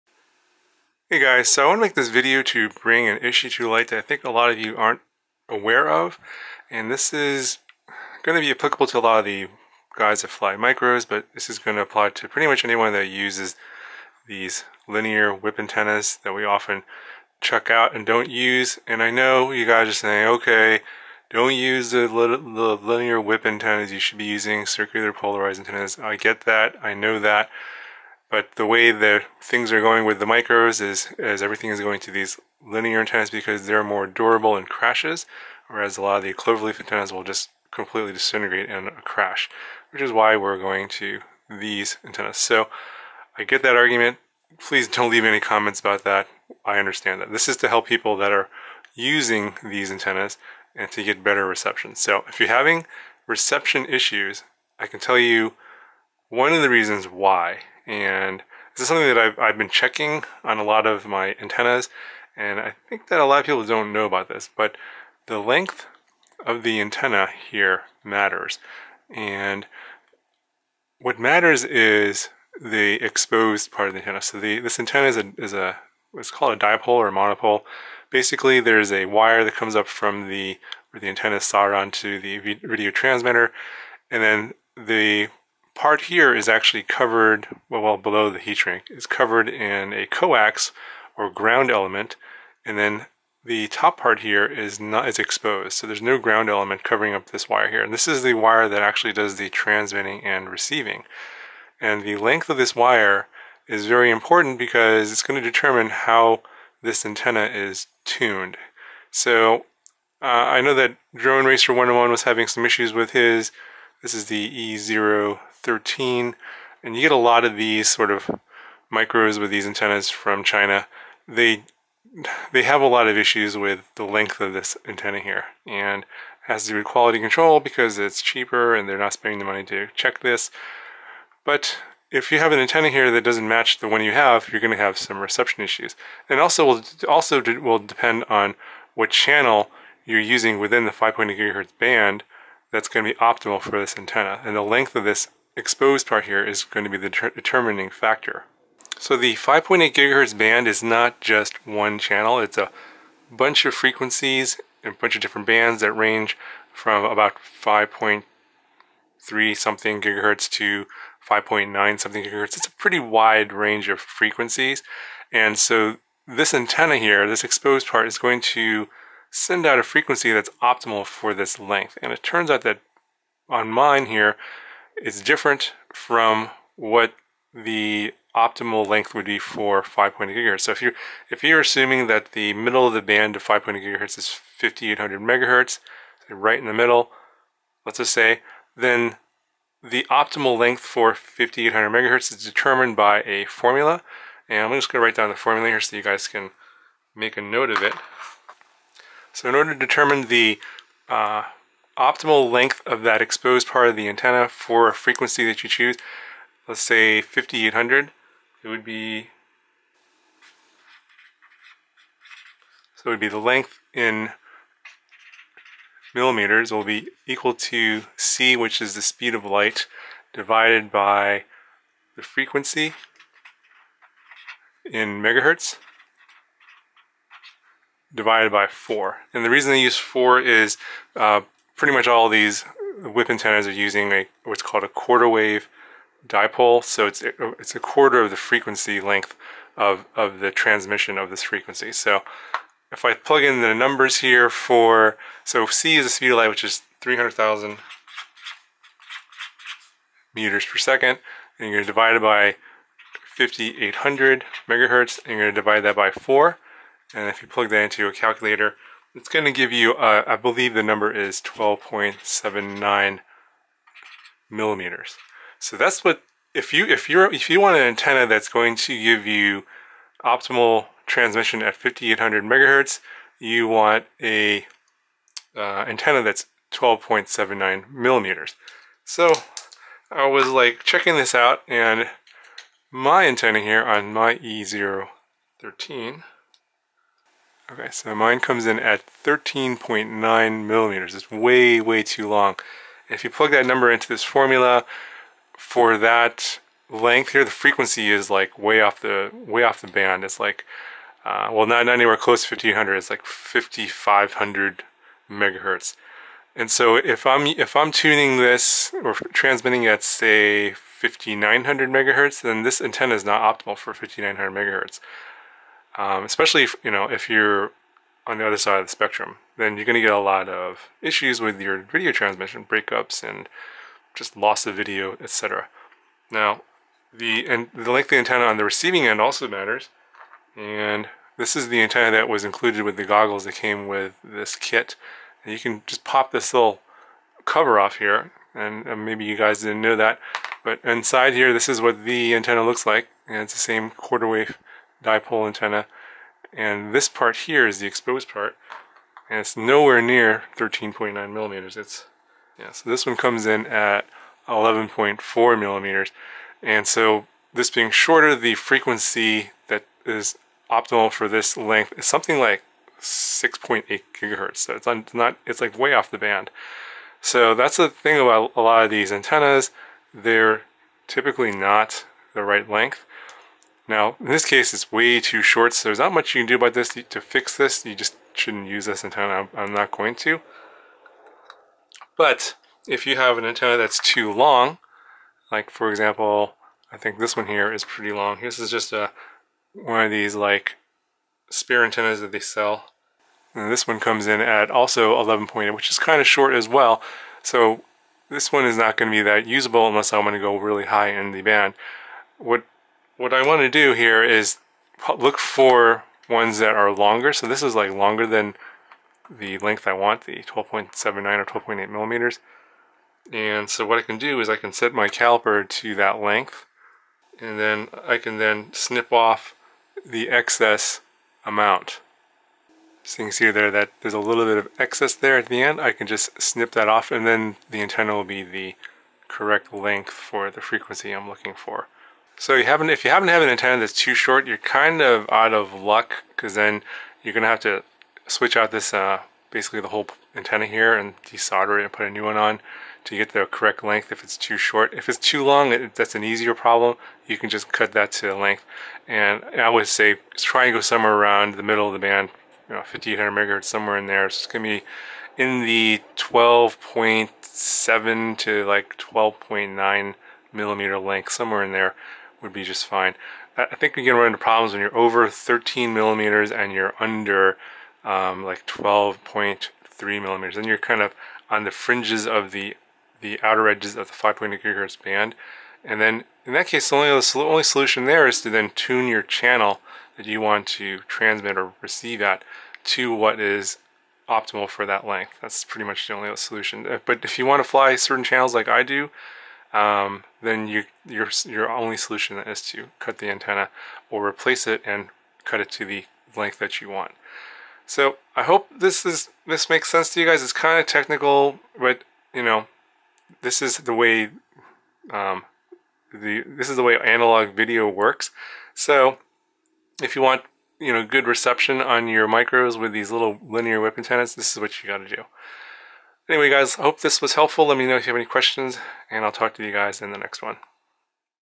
Micro Drone PSA 🚁 Fix Your Antennas! ⭐⭐⭐
Many of you may not know about how badly tuned linear antennas can lead to poor video reception.  I'll walk you through the nitty gritty!

⭐ Note - I should have stated 300,000 km per second, NOT meters per second for all you science sticklers!!!

▼ RC Gear
Turnigy Evolution